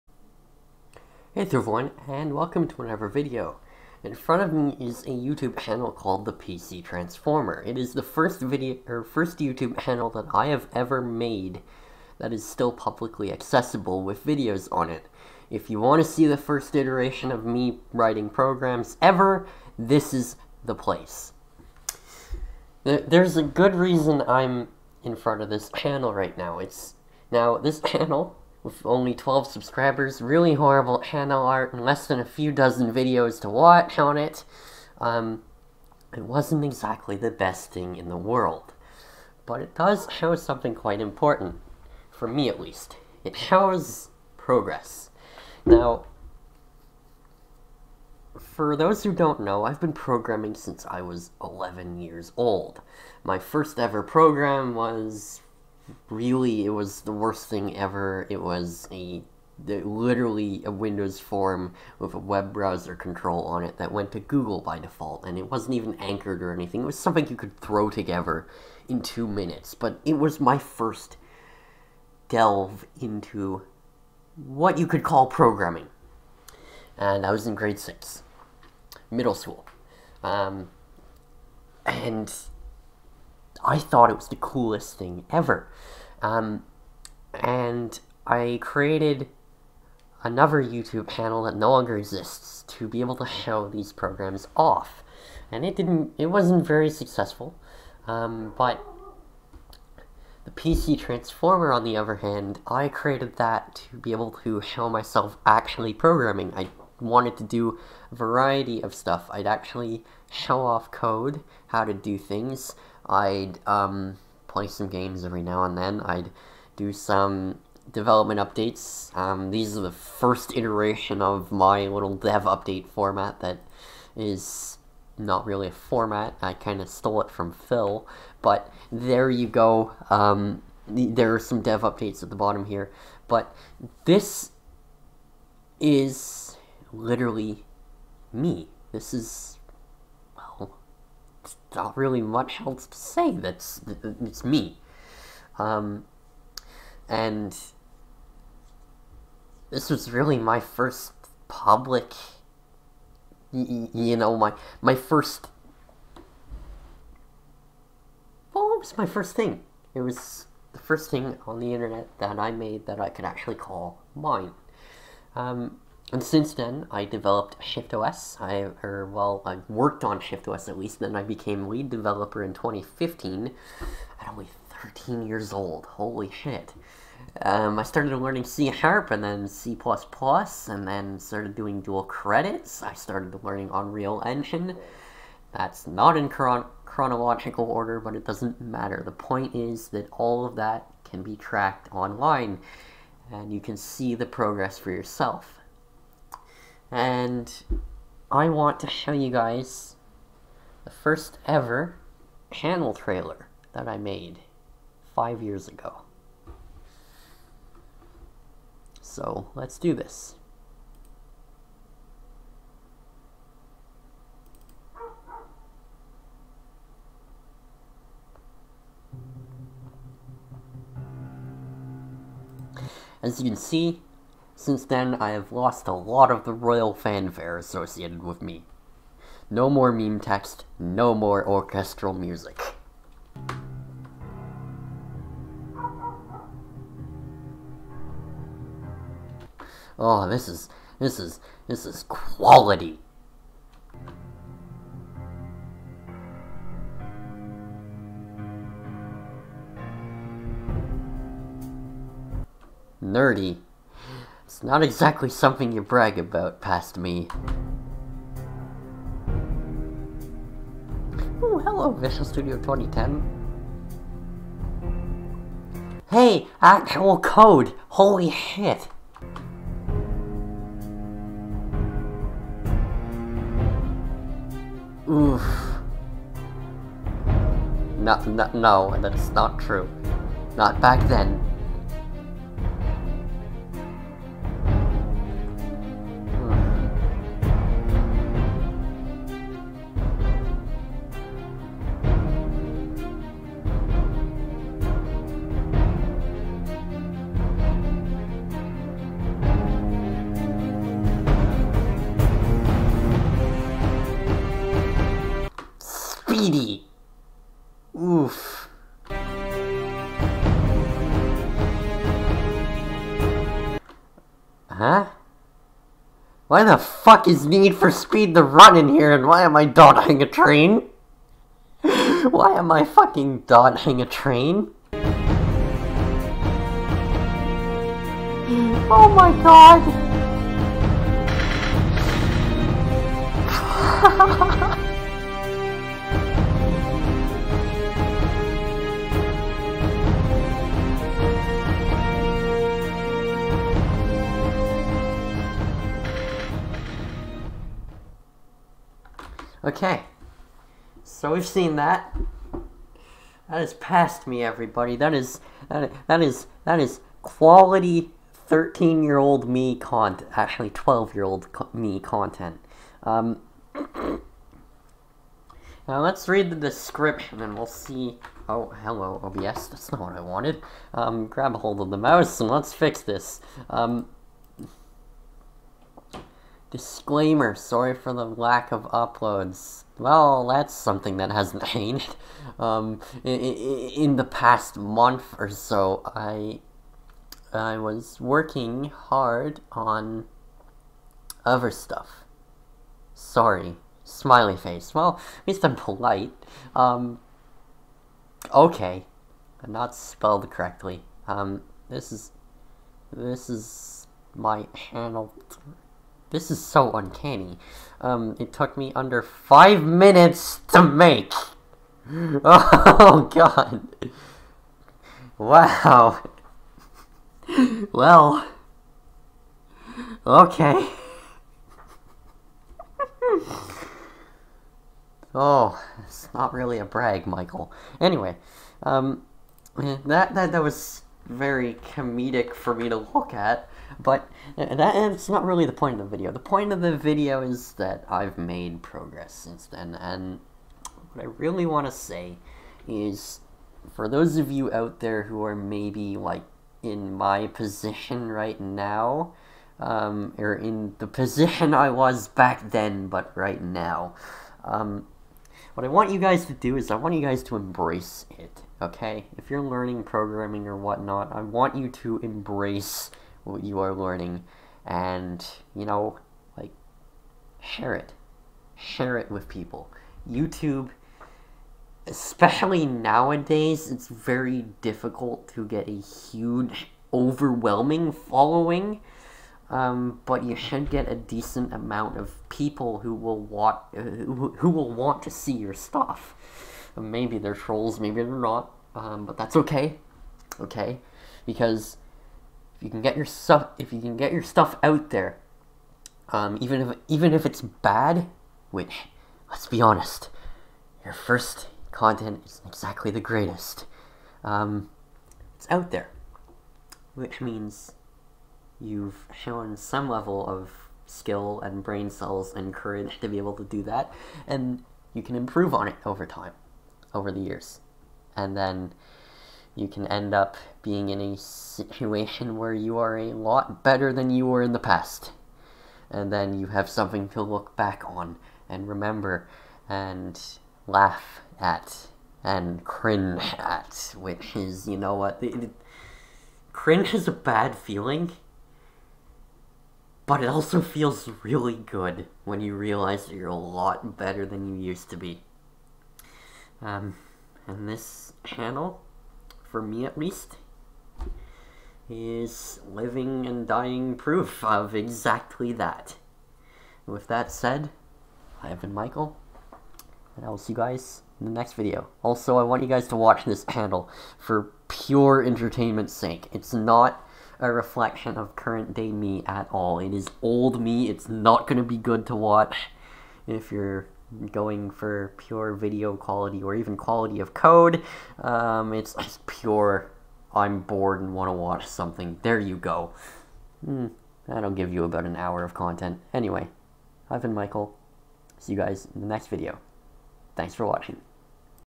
A Sense of Progress - Looking at my oldest public YouTube channel
Hey there.  In this video I decided to take a look back at my 5-year-old YouTube channel, ThePCTransformer - the channel that started it all for me.  With only 12 subscribers, not a lot of videos, and blinding channel art, it's not the most fun channel to look at or watch and it certainly wasn't successful.  But, for me, it gives me a sense of progress - being able to see living proof of how much I've improved over the past 5 years.

I spend a lot of time on this channel laughing at and making jokes at the expense of my (and others') really old content. But at the end of the day, it's all in good fun and never meant to put down anyone's (not even my own) creations.

Your first ever creations when learning a new talent/skill are extremely important and something you should genuinely be proud of.  They may not be the best quality - and 5 years later you'll be cringing at it like crazy - but embrace it. Show it off. Show off anything you make. Because then you'll have a record to look back on years later, something you can cringe at after you've made years worth of progress on that skill. And that type of cringy "whyyy did I do this" feeling, once you realize that sense of progress, will turn into an amazing feeling.